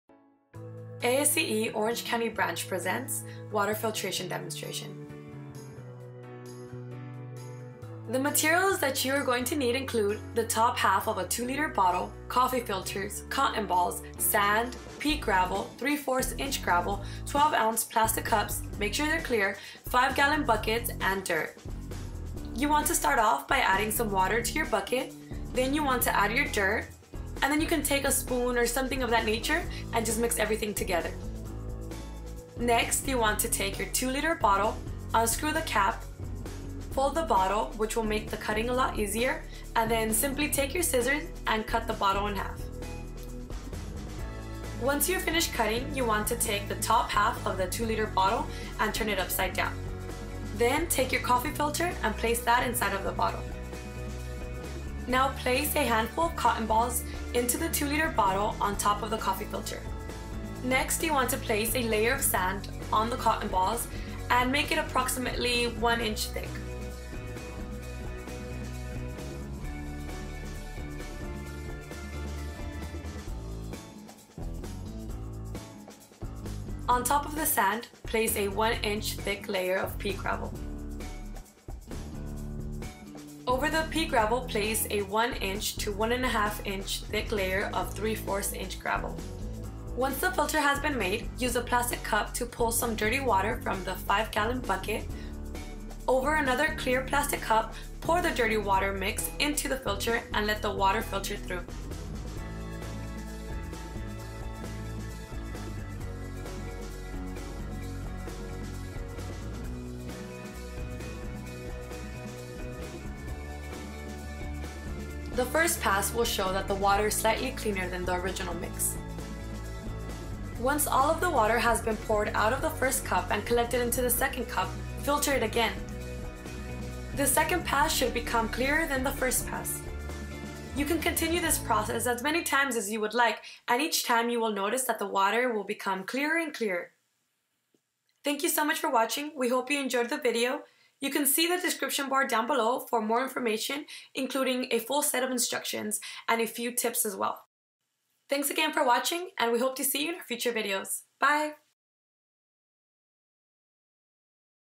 Water Filtration Demonstration
Water Filtration for Kids

Items Needed:
• Top ½ of a 2-L bottle (or automotive funnel).
• Coffee Filters (or shop towels)
• Cotton Balls
• Sand
• Pea Gravel
• ¾” Gravel
• About 12-12oz plastic cups (clear)
• 4 or 5 5-gallon buckets (to hold dirty water, filtered water, and materials)
• Dirt (this will need to be mixed with water and kept in a 5-gallon bucket)

Instructions:
1. Mix dirt with about 3-4 gallons of water to make a testing solution.
2. Place coffee filter into 2-L bottle.
3. Place a handful of cotton balls into the 2-L bottle on top of the coffee filter.
4. Place a layer of sand on the cotton balls approximately 1” thick.
5. On the sand, place a 1” thick layer of pea gravel.
6. On the pea gravel, place a 1” to 1-1/2” thick layer of ¾” gravel.
7. Once the filter has been made, use a plastic cup to pull some of the dirty water mix from a 5-gallon bucket.
8. Over another clear plastic cup, pour the dirty water mix into the filter and let the water filter through.
9. The 1st pass will show that the water is slightly cleaner than the original mix.
10. Once all the water has been poured out of the 1st cup and collected in the 2nd cup, filter it again…
11. Put the 2-L bottler over the 1st cup (emptied) and pour the mix from the 2nd cup through the filter.
12. The 2nd pass should become clearer than the 1st pass.
13. Continue this process a few times and each time, the water will become clearer.

A few thoughts:
• Purchasing everything should cost no more than $50, depending on how many filters you want to make.
• 2-L bottles are a better way to go than a funnel because they are clear…funnels are not. If you decide to go with funnels, consider buying the long neck funnels.  They are not as wide and also have a smaller opening.  You will need a 2-L bottle for each simultaneous demonstration you want to conduct.
• Shop towels function better than a coffee filter because they are bigger and much stronger, but they do cost more. (about $10 for a large box @ Home Depot).
• The rock can be purchased at Home Depot and placed in a bucket…Rock is not very expensive, but it is sold in 50-lb bags.  It will last for many demonstrations.